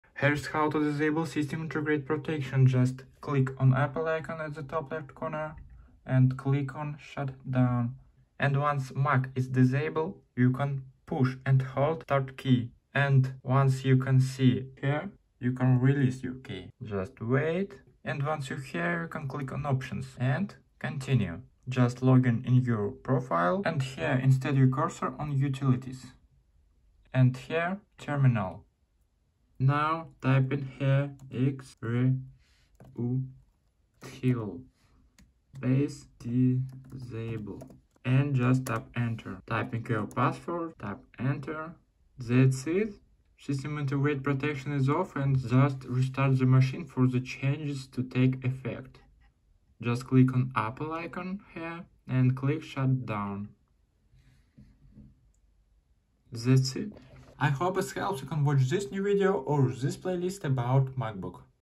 How to Disable SIP on Mac | Step-By-Step Disable System Integrate Protect MacOS
0:00 Intro
0:05 How to Disable SIP on Mac
0:55 end

In this short video I'll show how to disable SIP on your MacOS.

Simplified Technology tells you all you need to know about tech: gadgets, how-to's, gaming, science, digital culture, and more.